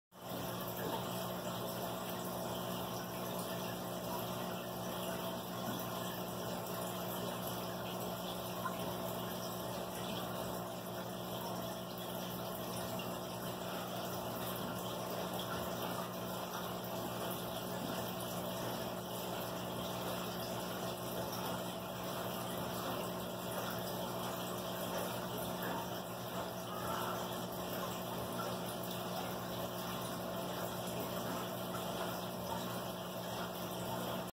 Black swordtails and glo tetras
Glo tetras and black swordtails in the sukken ship and plane tank